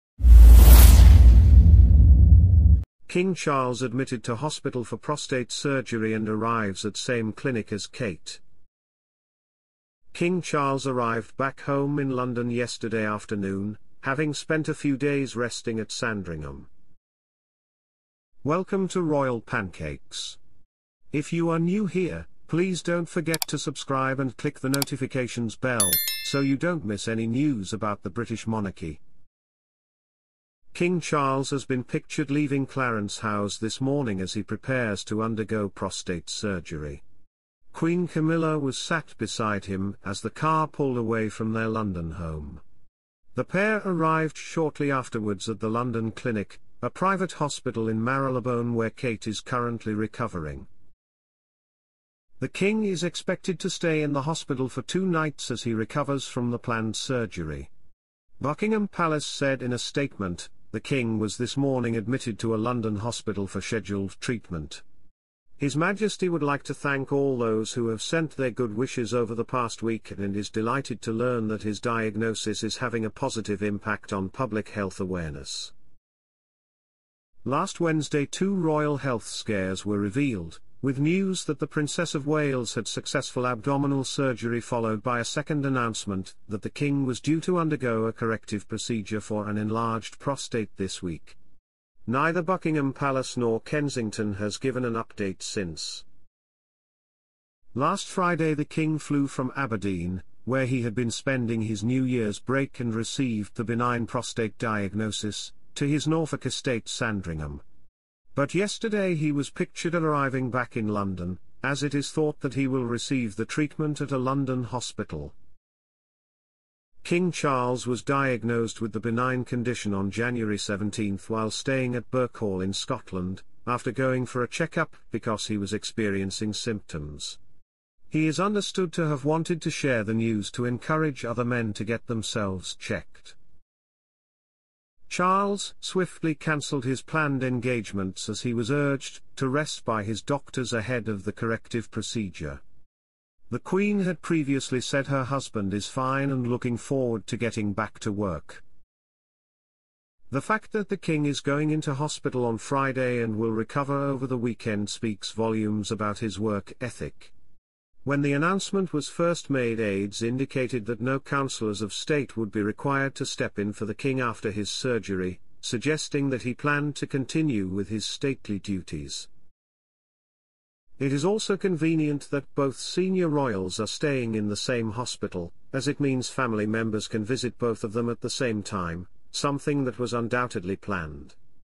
POISONED!🛑 Camilla Kicked out of King Charles hospital after deadly poison seized from her handbag
Queen Camilla smiles warmly as she heads to hospital to visit King Charles.

The Queen was pictured being driven to hospital as King Charles spends his third day recovering after his operation.

Queen Camilla has been spotted smiling warmly as she headed to dote on her husband King Charles in hospital three days after his successful operation.

The Queen also went to see her husband yesterday at a similar time, after he was admitted to the London Clinic on Friday to undergo surgery for an enlarged prostate.

A statement from Buckingham Palace read at the time: "The King was this morning admitted to a London hospital for scheduled treatment. "His Majesty would like to thank all those who have sent their good wishes over the past week and is delighted to learn that his diagnosis is having a positive impact on public health awareness."

On January 17 it was confirmed that Charles would be entering hospital for treatment for his prostate condition and he has been open with his diagnosis in order to encourage others to be aware.

The King is expected to be discharged soon and will take some time off from his royal duties to recover, with Queen Camilla set to take the reins at the helm of the Firm.

The London Clinic in Marylebone is also where Princess Kate is recuperating after she underwent abdominal surgery on January 16, with the new coming as a suprise to everyone. The following day Kensington Palace confirmed the operation had been a success and that the Princess would be following medical advice by not returning to royal duties until after Easter.

Before his own operation the King popped in to his visit his daughter-in-law, who has received regular visits from Prince William but none yet so far from Prince George, Princess Charlotte and Prince Louis.

Queen Camilla arrived at the clinic with her husband on Friday in what was dubbed an "unusual" move, as royals in the past were generally not accompanied into hospital by their spouse.

ITV's royal editor Chris Ship noted the change in precedent, writing on X: "Queen Camilla has gone into the hospital with King Charles. Whilst this might seem normal for most families, it is very unusual for the Royal Family to accompany their spouse like this (the late Queen and Prince Philip always went into hospital on their own.)"

As her husband recovers Queen Camilla will be even more heavily relied upon to carry out royal engagements and represent the Firm at home. Royal expert Richard Fitzwilliams said: "I think it is clear that with three senior working members of the Royal Family temporarily unable to carry out engagements, that the Queen will both continue her planned engagements and help King Charles recuperate after his operation.

"It is worth remembering that she is extremely tough and for many years remained silent when she was the object of so much criticism. She will help the King, who is her soulmate, regain his strength."princeharry